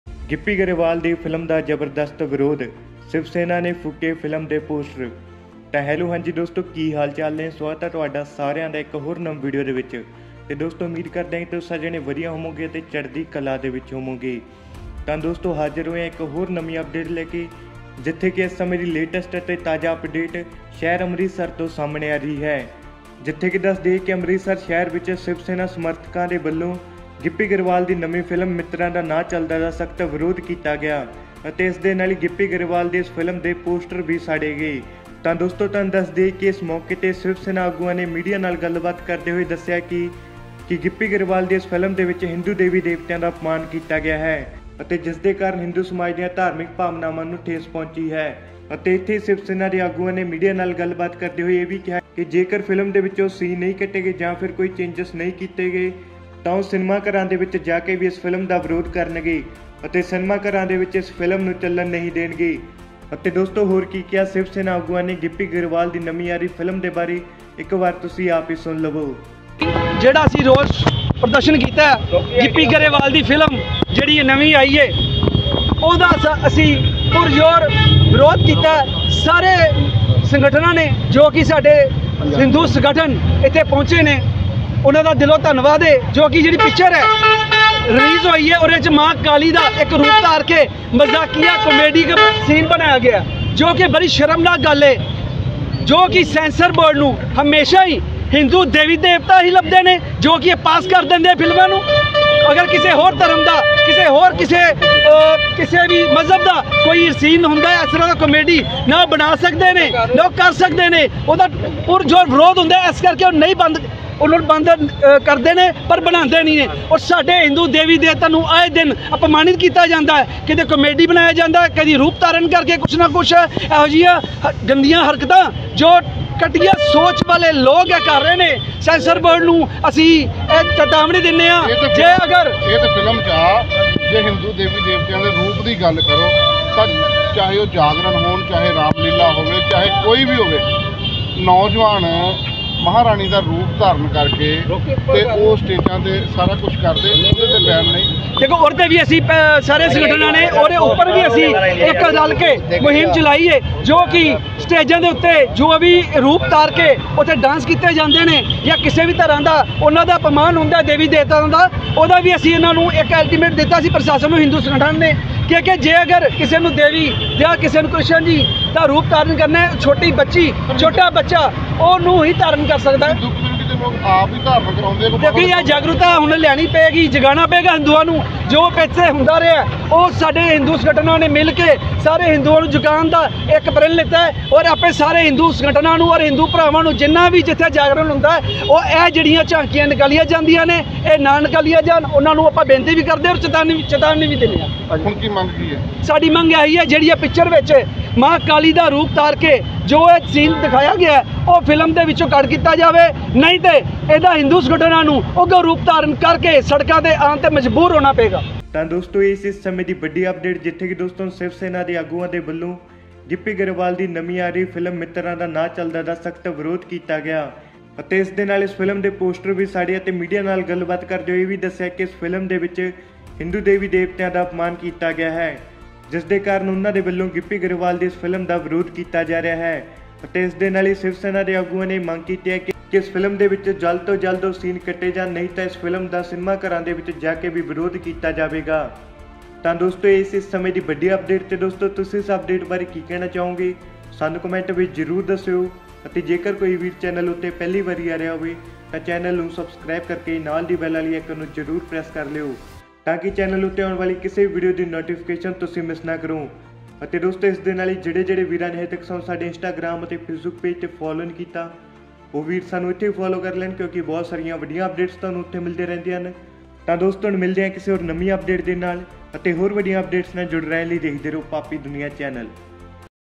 Gippy Grewal New Movie Boycott By Shiv Sena | Mitran Da Naa Chalda Movie Boycott Amritsar News
Paapi Dunia